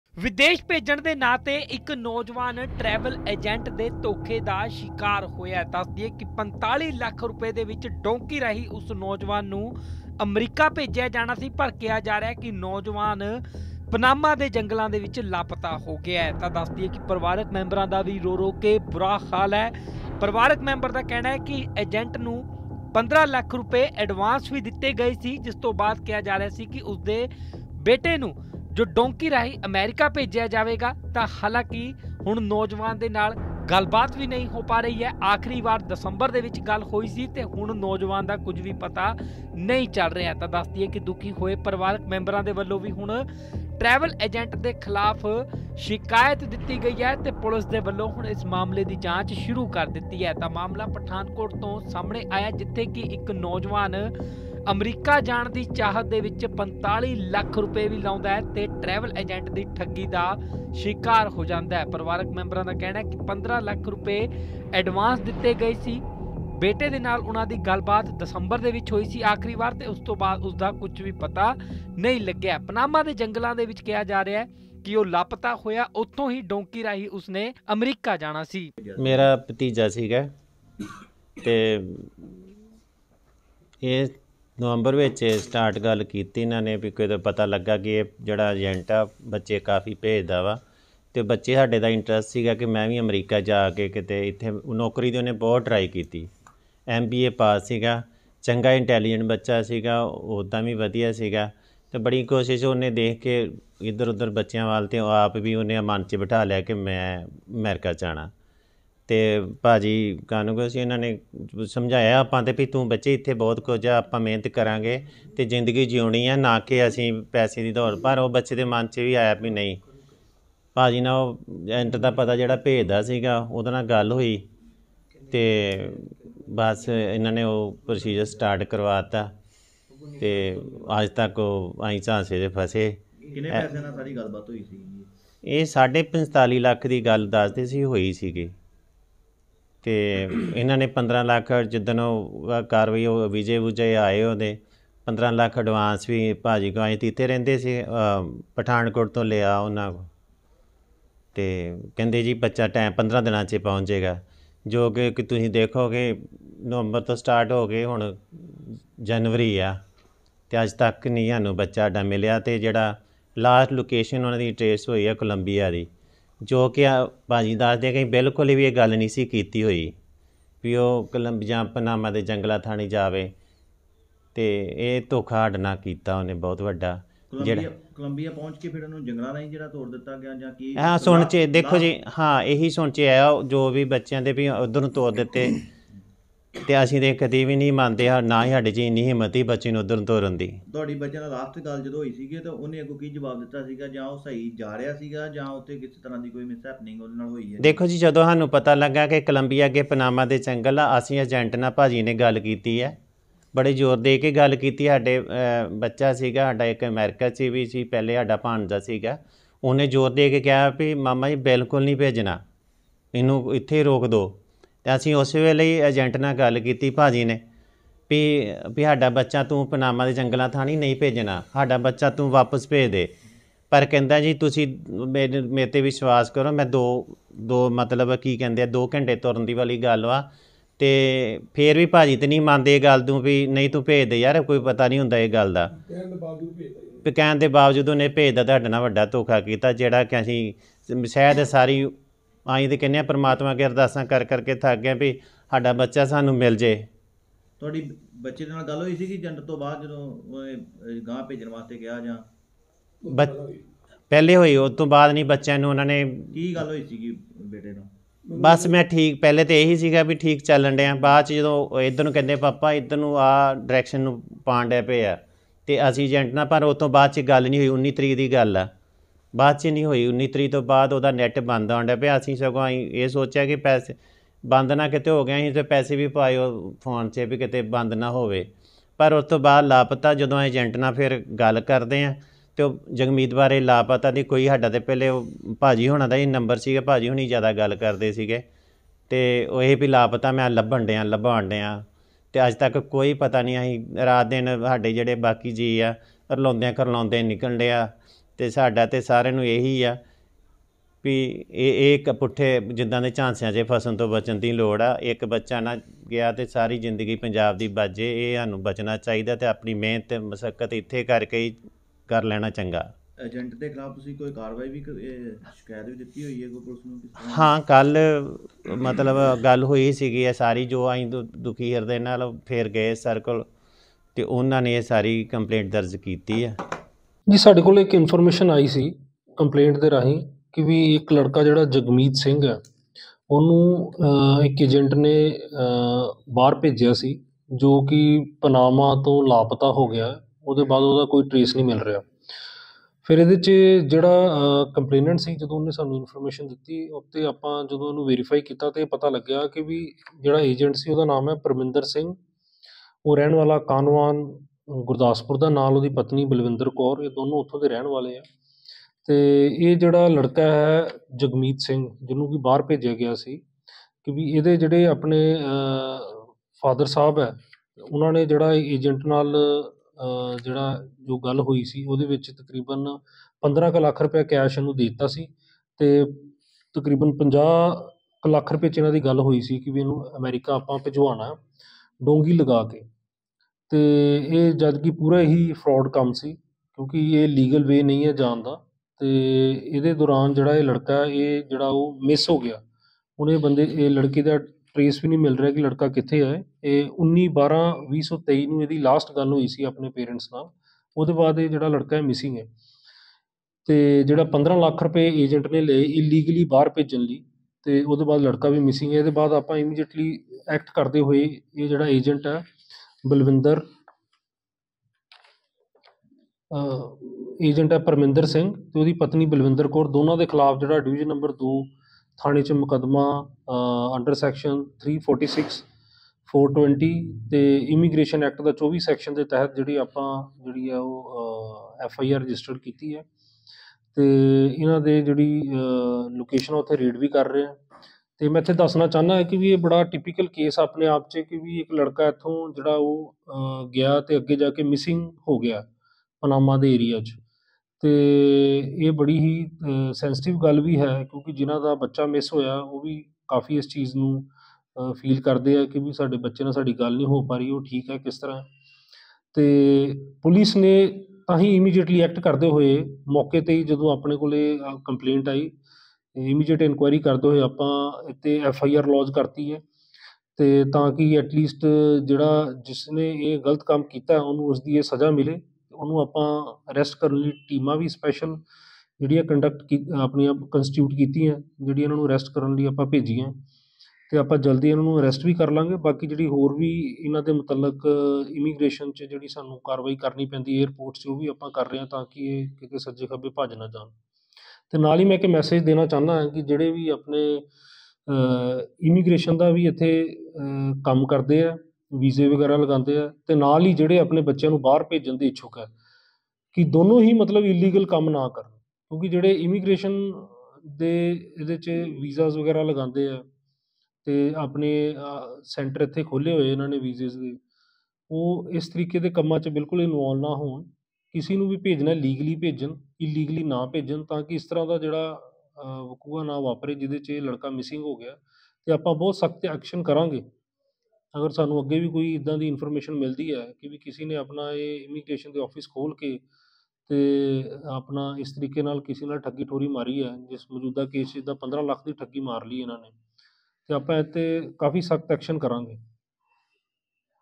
45 ਲੱਖ ਲਾ ਕੇ ਵੀ ਨੌਜਵਾਨ ਨਹੀਂ ਪਹੁੰਚਿਆ ਅਮਰੀਕਾ, ਡੌਂਕੀ ਲਾ ਰਿਹਾ ਨੌਜਵਾਨ ਜੰਗਲਾਂ 'ਚ ਹੋਇਆ ਲਾਪਤਾ
ਇਹ ਮਾਮਲਾ ਪਠਾਨਕੋਟ ਤੋਂ ਸਾਹਮਣੇ ਆਇਆ ਹੈ ਜਿੱਥੇ ਇੱਕ ਪਰਿਵਾਰ ਨੂੰ ਵਿਦੇਸ਼ ਭੇਜਣ ਦੇ ਨਾਂ 'ਤੇ ਟਰੈਵਲ ਏਜੰਟ ਨੇ ਠੱਗੀ ਮਾਰੀ ਹੈ। ਦੱਸ ਦਈਏ ਕਿ  ਨੌਜਵਾਨ ਨੂੰ ਕਰੀਬ 45 ਲੱਖ ਰੁਪਏ 'ਚ ਅਮਰੀਕਾ ਭੇਜਿਆ ਜਾਣਾ ਸੀ ਪਰ ਕਿਹਾ ਜਾ ਰਿਹਾ ਹੈ ਕਿ ਨੌਜਵਾਨ ਪਨਾਮਾ ਦੇ ਜੰਗਲਾਂ ਦੇ ਵਿਚ ਲਾਪਤਾ ਹੋ ਗਿਆ ਹੈ।